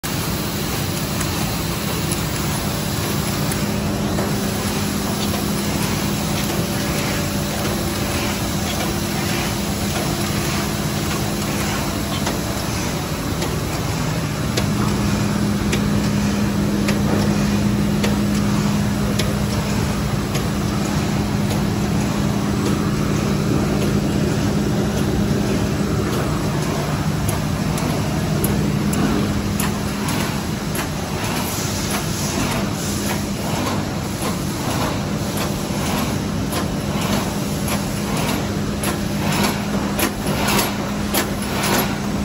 BOBST SPanthera 106 LE, 2005, specially for IML, films and board
ANT Maschinenhandel UG, Germany presents:

BOBST SPanthera 106 LE specially for IML, films and board

Max. sheet size 1060 х 760 mm
Min. sheet size 400 х 350 mm
Speed 9000 sh/h
non-stop feeder Processing
Pre make ready table Centerline System
CUBE system
Chase Changer
cutting chases (quick lock) Antistatic
Stripping Unit
Non-stop delivery